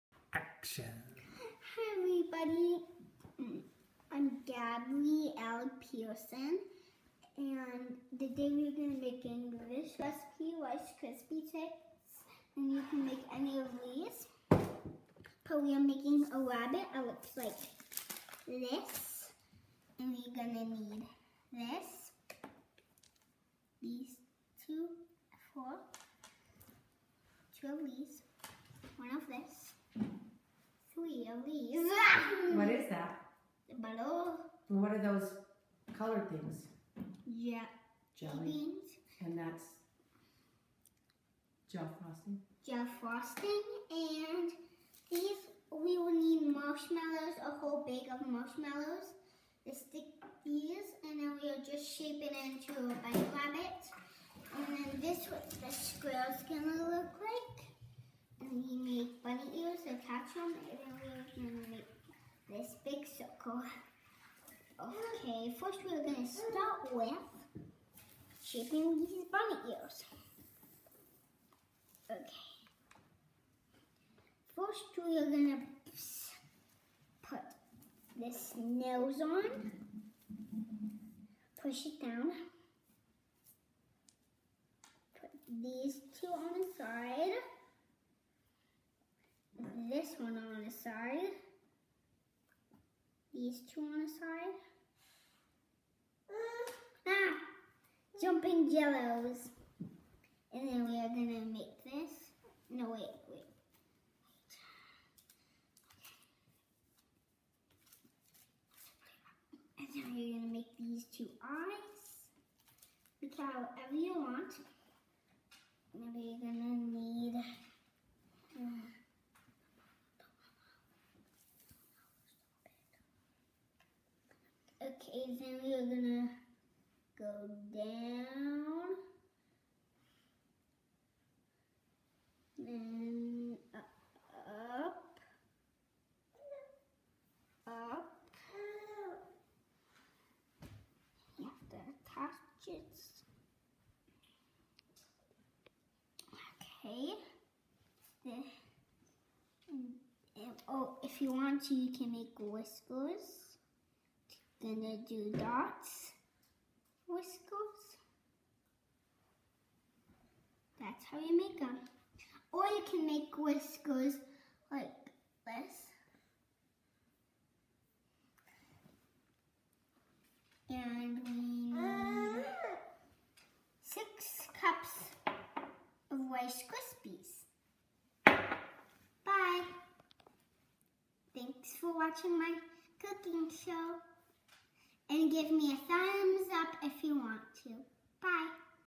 Chef Gabbi Makes Rice Krispie Easter Bunnies
Another exciting cooking show from Chef Gabbi!!